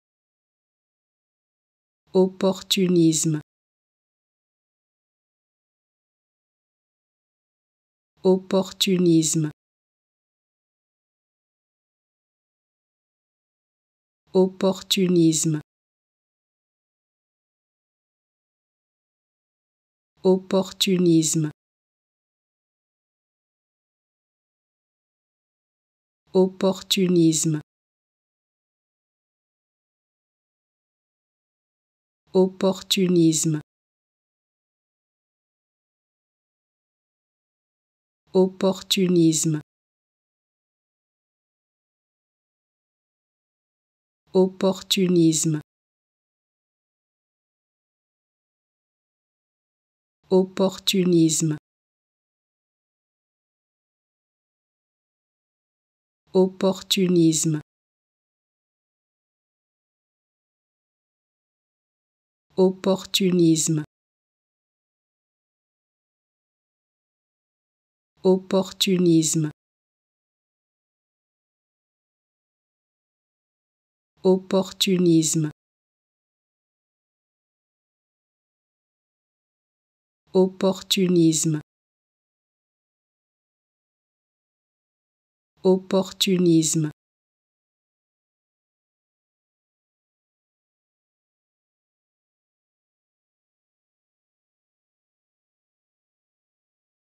How to Pronounce ''Opportunisme'' (Skydiver) Correctly in French
Learn how to say and properly pronounce ''Opportunisme'' in French with this free pronunciation tutorial.